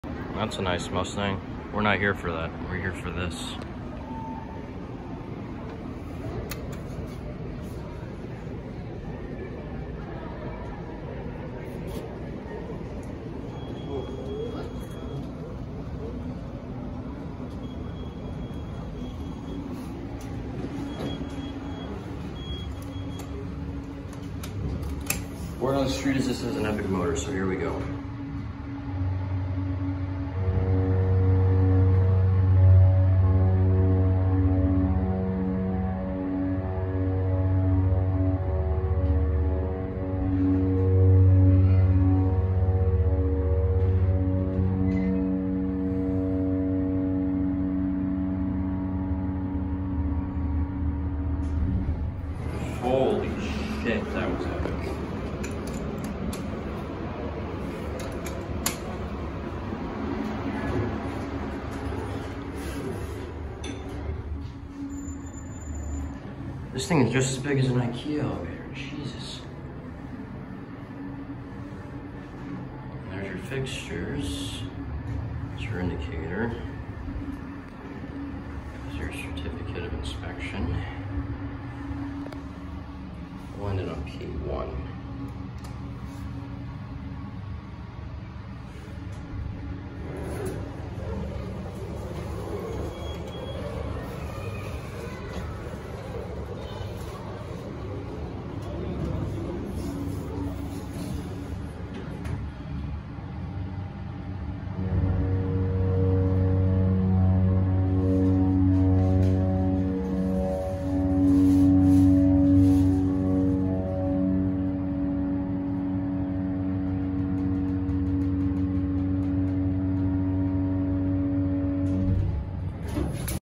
Epic 1997 MontgomeryKONE Hydraulic Parking Elevator @ Museum of Science & Industry - Chicago, IL
Recorded on November 12th, 2023.

     This is one of the parking elevators at the Museum of Science & Industry in Chicago, IL. They are both MontgomeryKONE's installed around 1997 or so when this underground parking garage was eventually completed in July of 1998. These elevators I have not ridden since the summer of 2017 and today I was pleased to find out that they are still running strong 6 years later after I was last here.

|Elevator Facts|
+ Manufacturer/Make: MontgomeryKONE
+ Installed: 1997
+ Interlock/Door Equipment: MAC
+ Fixtures: Innovation Security
+ Capacity: 8,000 lbs.
+ Floors Served: 4 (*P3-ST)
+ Type: Hydraulic